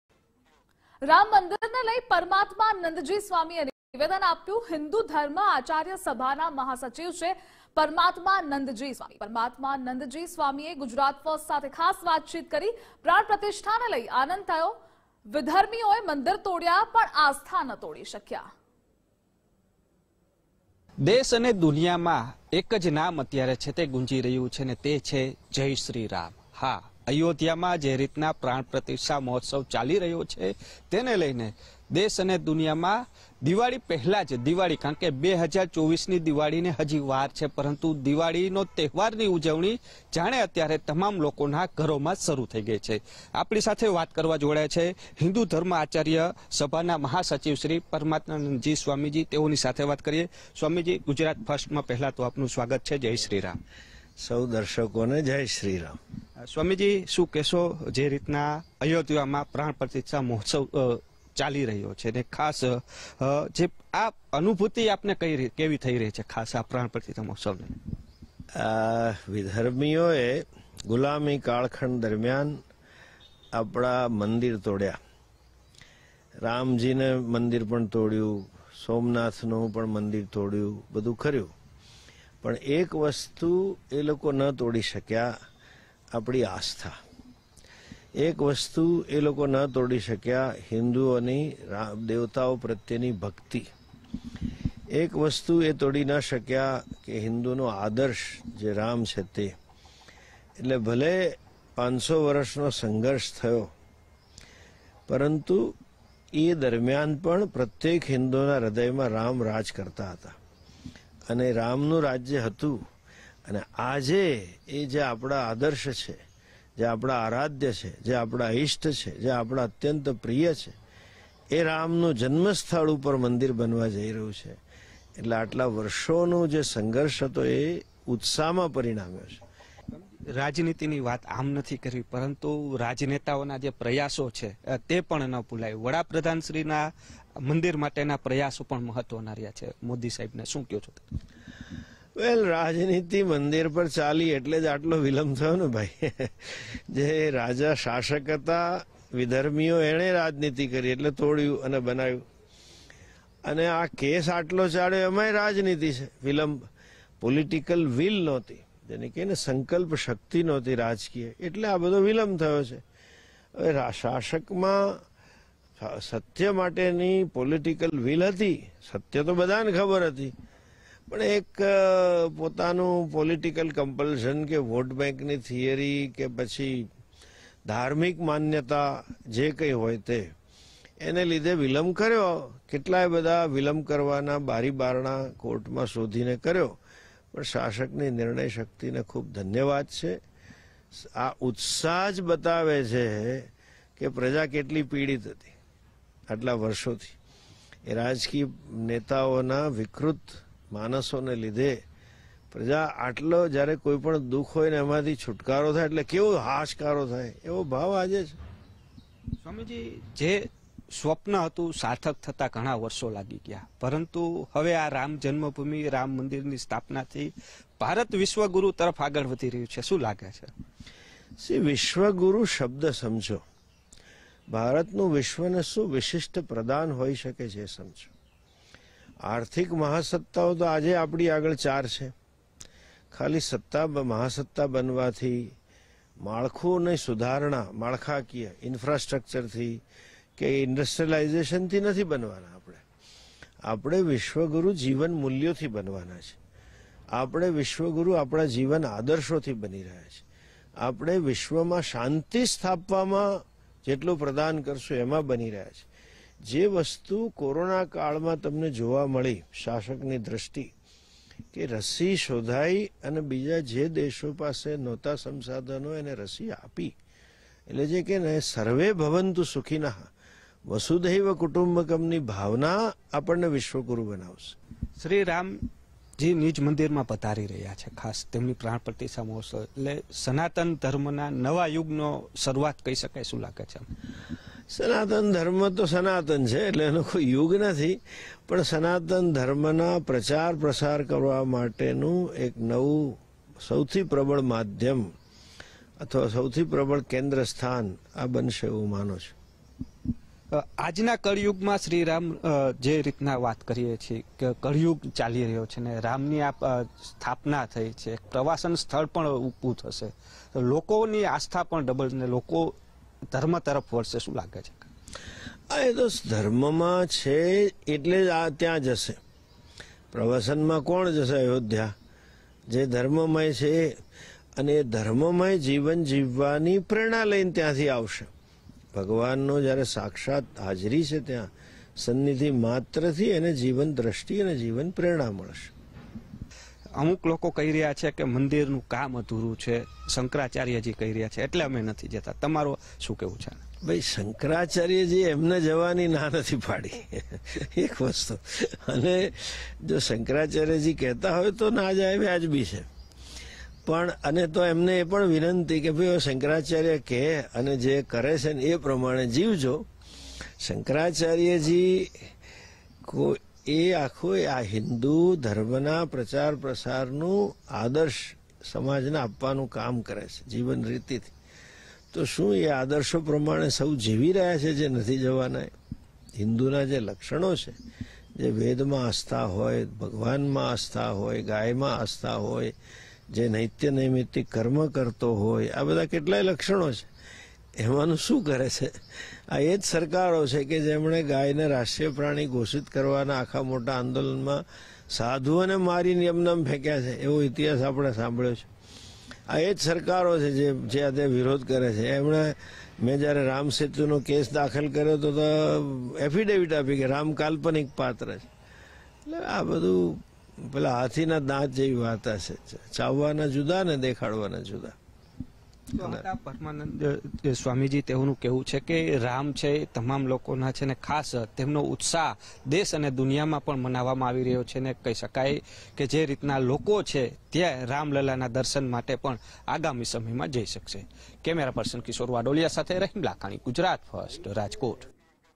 Ramotsav 2024: "વિધર્મીઓએ મંદિર તોડ્યા પણ આસ્થા ન તોડી શક્ય'': Parmatmanandji Swami | Gujarat First
"વિધર્મીઓએ મંદિર તોડ્યા પણ આસ્થા ન તોડી શક્યા": રામમંદિરને લઈ હિન્દૂ ધર્મ આચાર્ય સભાના મહાસચિવ પરમાત્માનંદજી સ્વામીએ આપ્યું નિવેદન